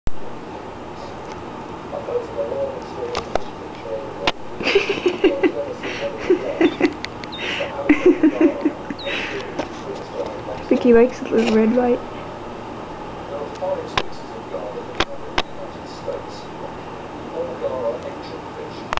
bird likes camera
I think it pecks at the camera cause of the red light on it. Because that's always what it aims for. This was taken some time ago. I just forgot to upload it untill now, lol.